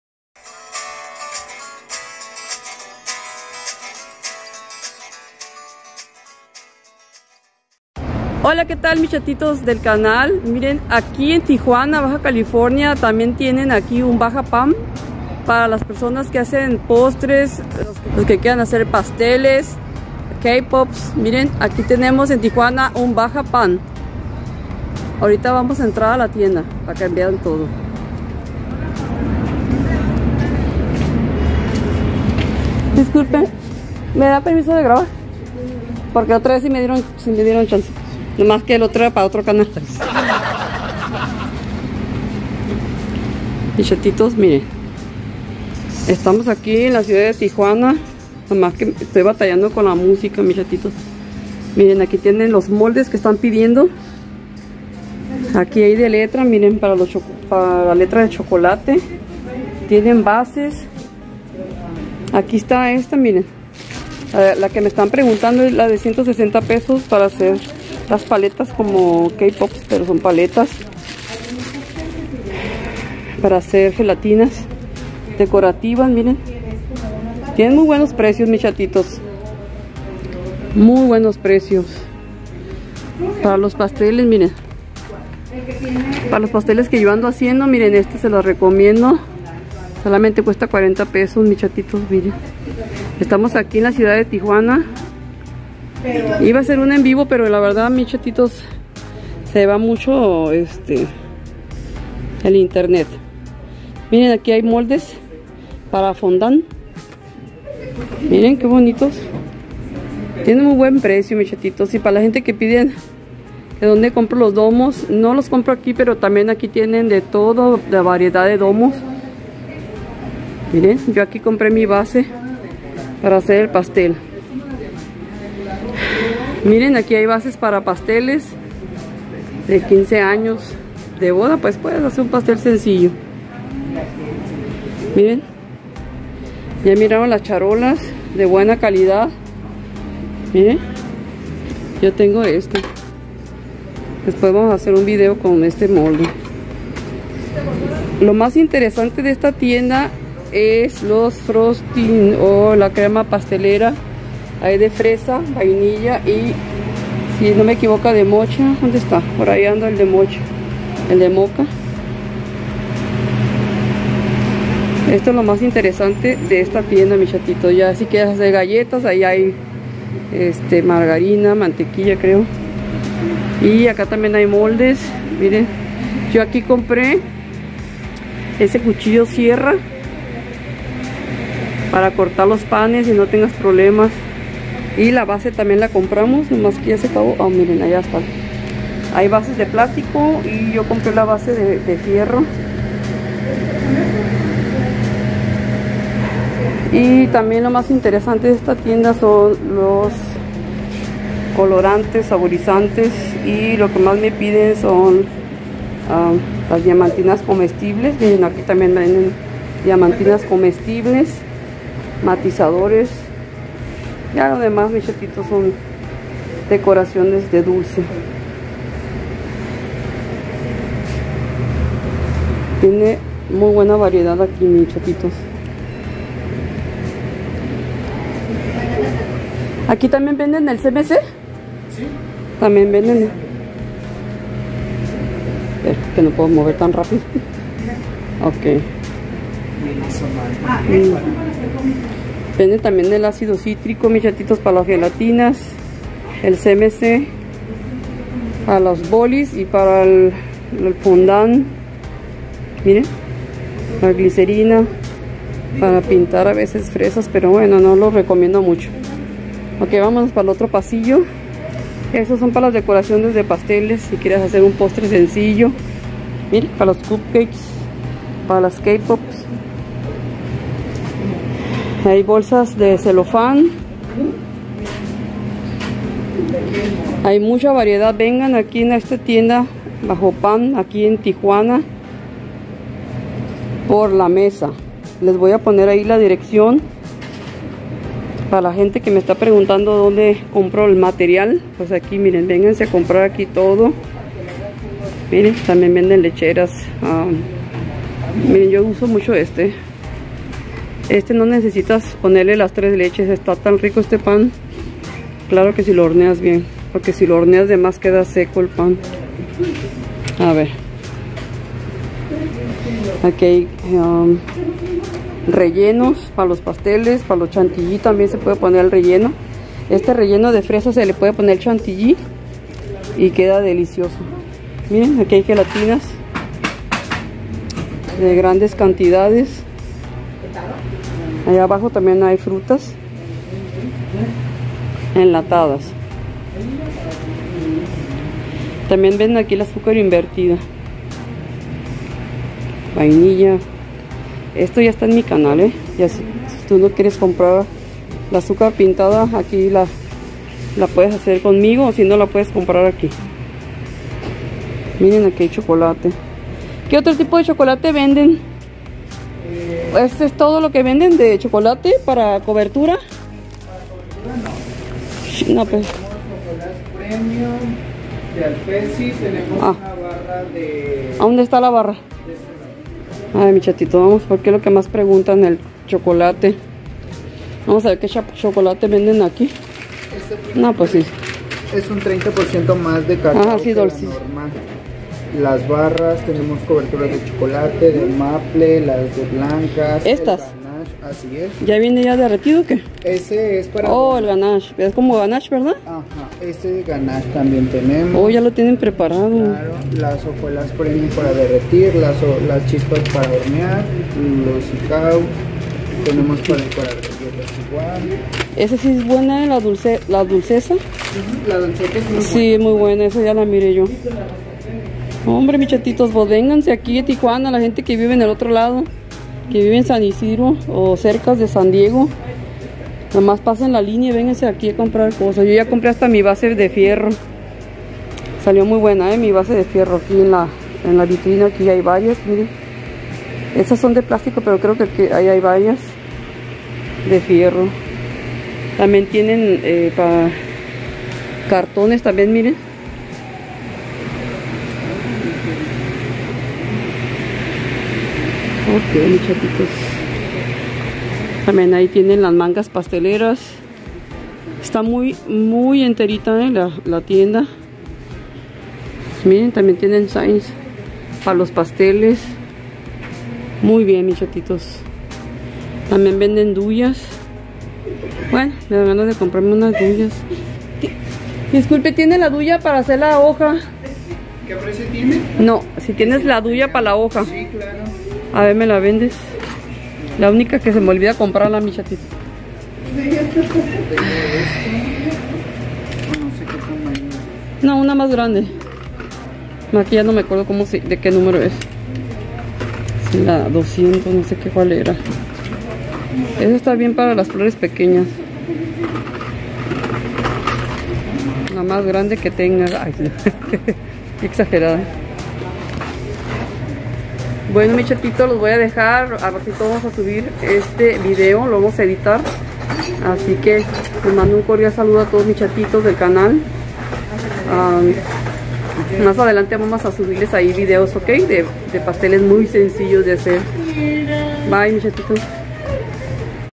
La Chatayou Visita BAJAPAN en Tijuana
Tienda Bajapan en tijuana
Dirección: Bluvd. Diaz ordaz
#14375 Inf. 6 plaza cedros

💛  Muchisimas  Gracias a todos mis chatitos por su gran visita a este tu canal

Te Dejo Estos Buenos Canales de Amigos y youtuberes reconocidos